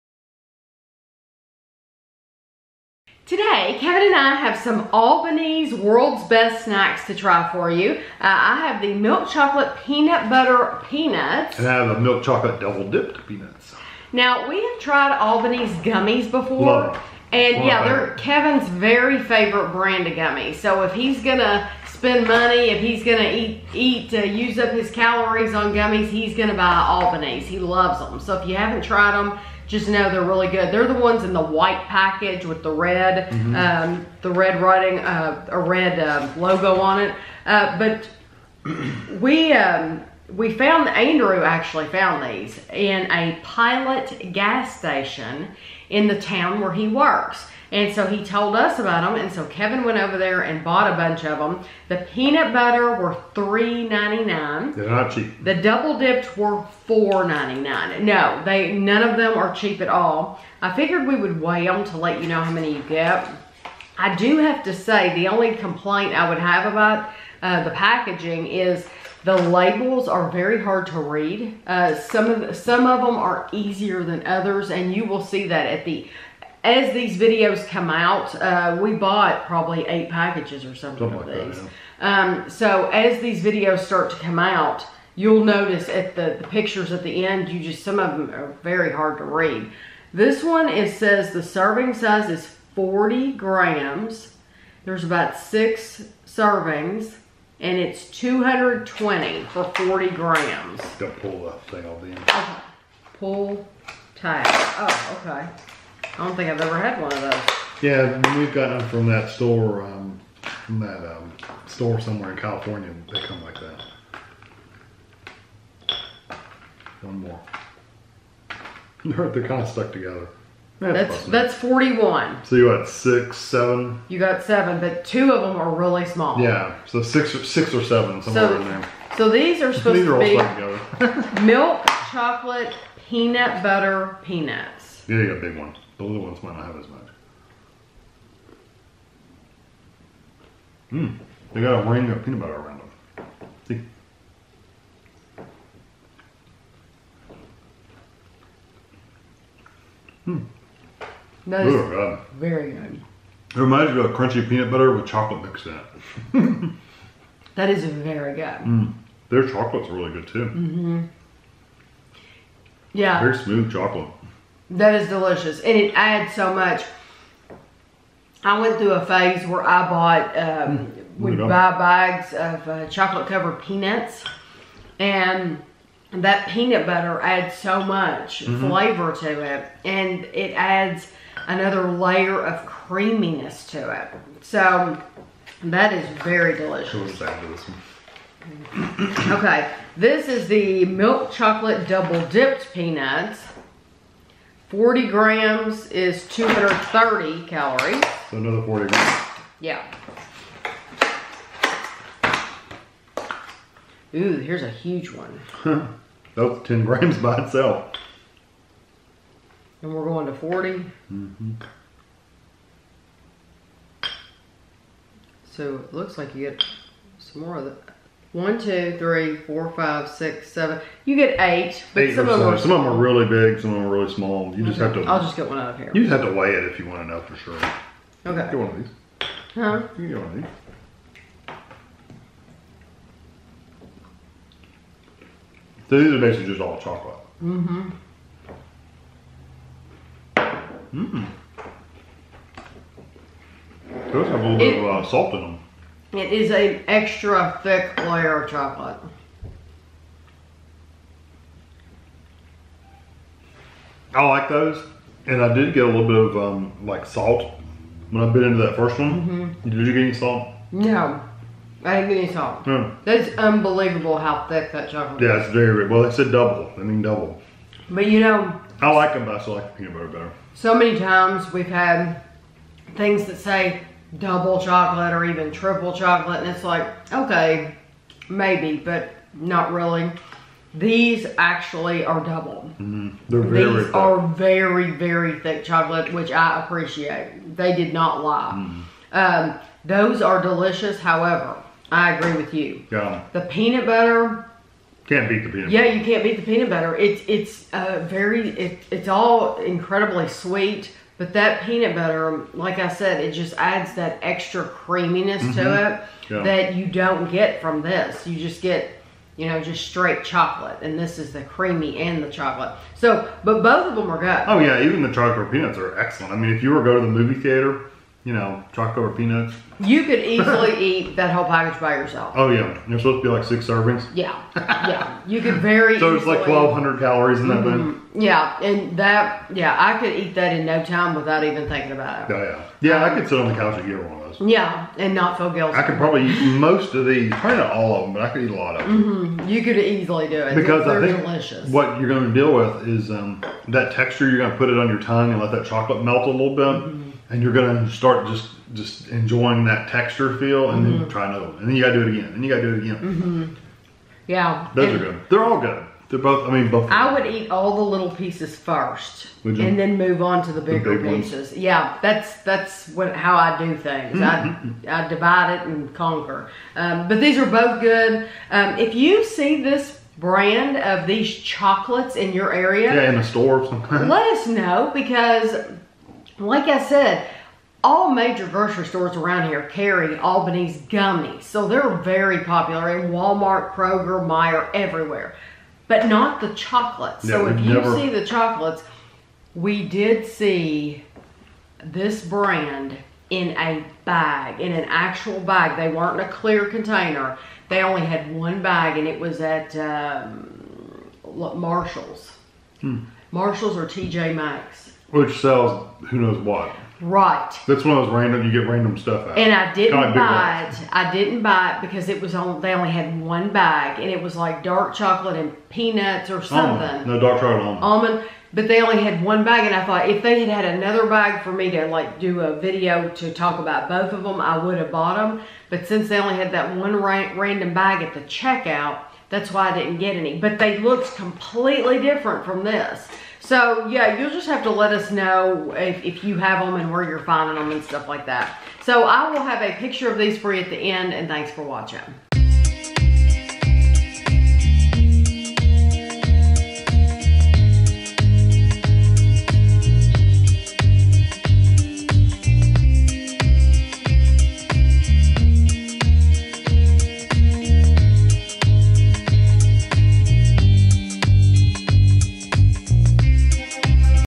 Albanese Milk Chocolate: Peanut Butter Peanuts & Double Dipped Peanuts Review
This is a taste test/review of the Albanese Milk Chocolate Peanut Butter Peanuts and the Milk Chocolate Double Dipped Peanuts.
* The Milk Chocolate Peanut Butter Peanuts were $3.99 at Pilot. (40g) = 220 calories
* The Milk Chocolate Double Dipped Peanuts were $4.99 at Pilot. (40g) = 230 calories